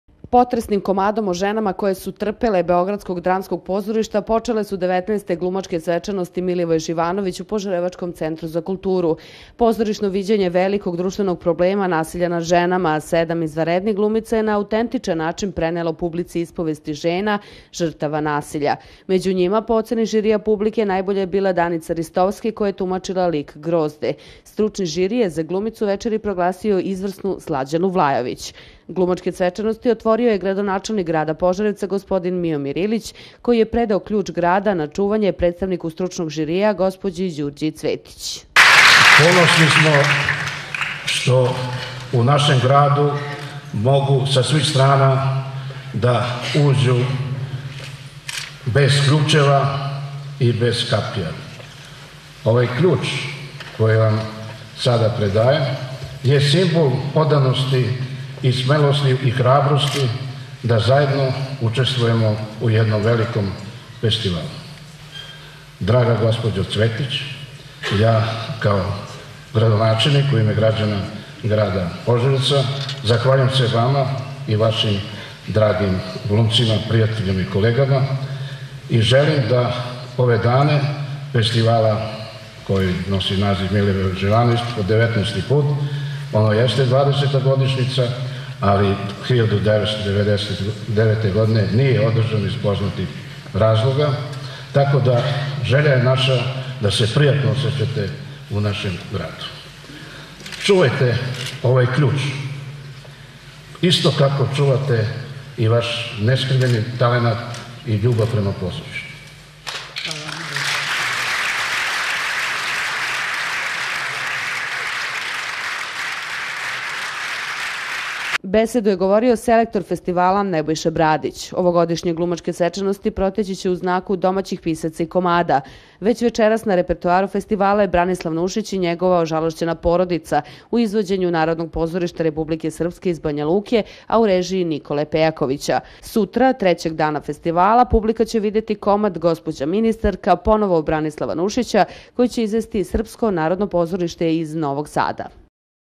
Info Sat_Pocele su XIX Glumacke svecanosti 'Milivoje Zivanovic"03_04_2014.
U Pozarevackom Centru za kulturu pocele su XIX Glumacke svecanosti 'Milivoje Zivanovic".